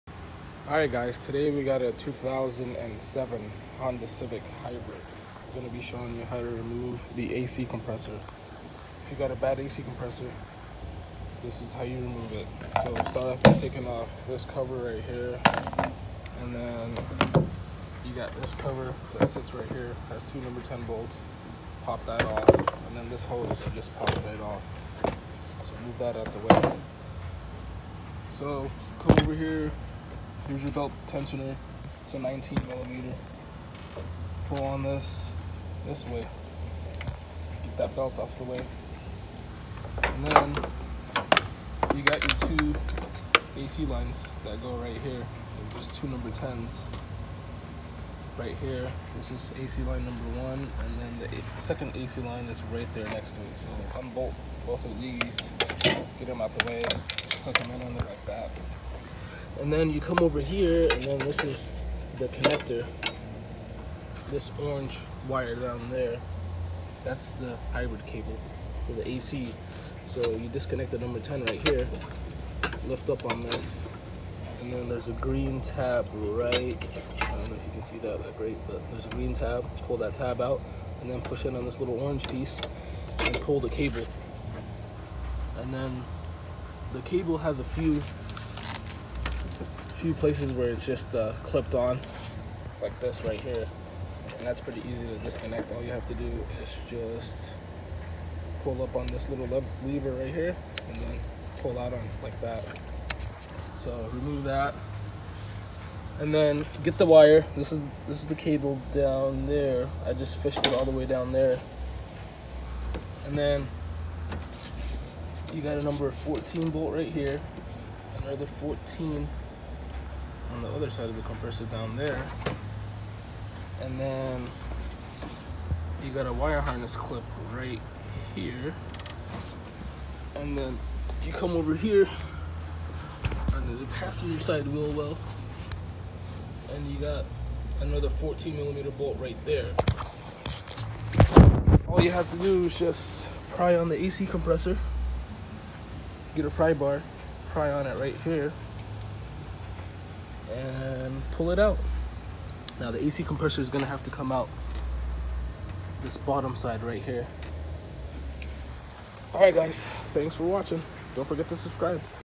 2006-2011 civic hybrid ac compressor removal
How replace an ac compressor on a civic hybrid